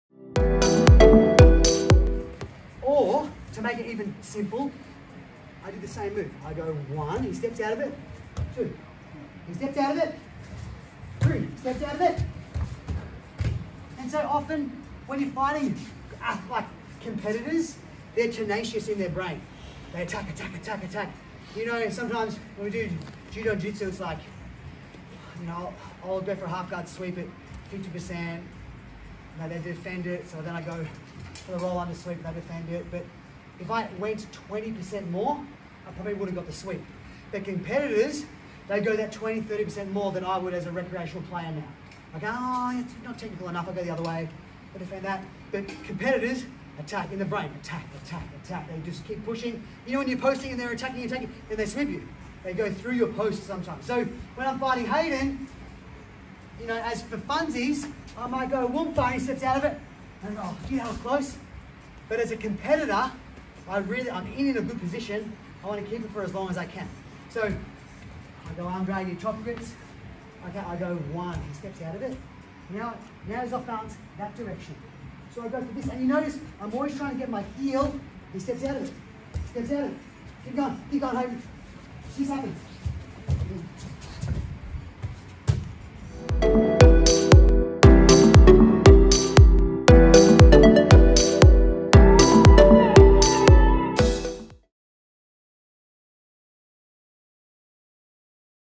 Seminar Clip Use a competitive mindset to throw or sweep more
A few thoughts on the fact that if you drive a bit harder you will land more attacks - but it takes a tenacious mind.

Have any questions? Want me to film a video?

Who am I? I am a 2008 Beijing Judo Olympian, 4th degree Judo black belt and BJJ Black belt under Felipe Grez